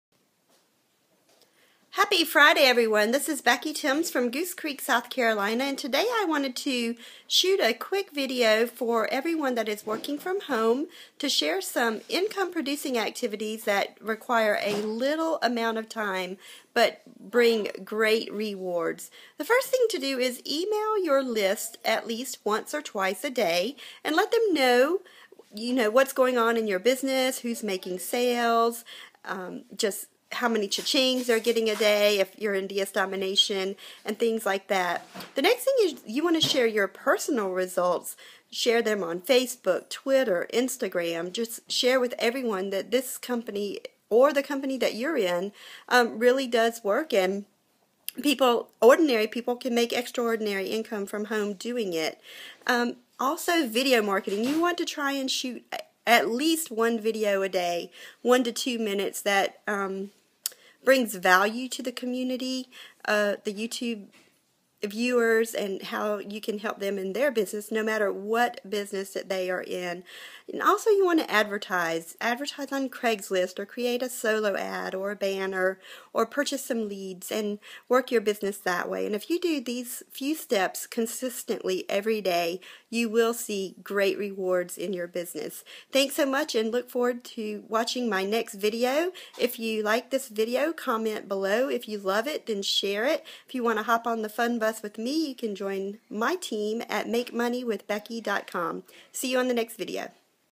Income producing activities
Examples of daily activities that can increase your income in your home based business.